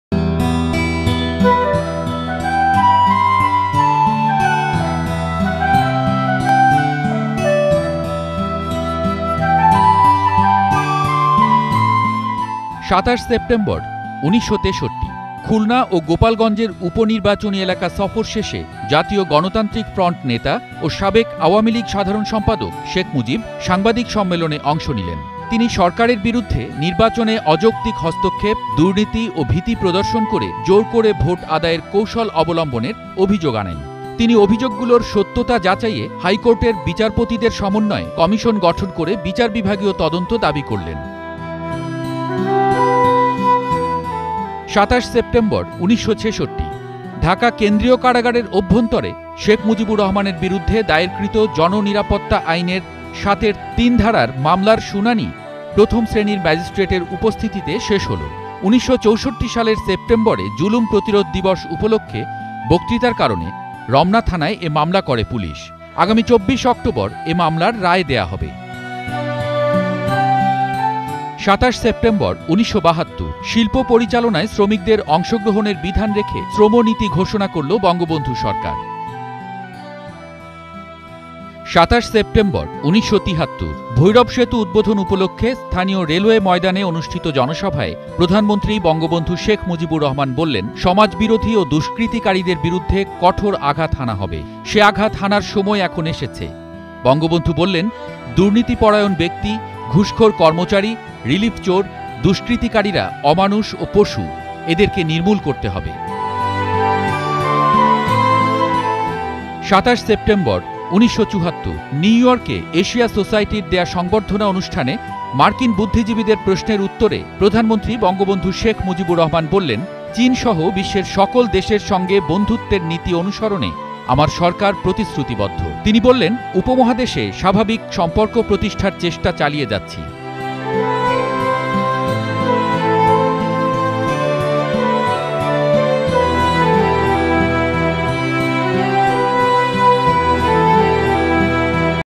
Timeline stories of The Father of the Nation Bangabandhu Sheikh Mujibur Rahman (1920-1975) - Sep 27
Here are some remarkable September 27th events from The Father of the Nation Bangabandhu Sheikh Mujibur Rahman's lifetime.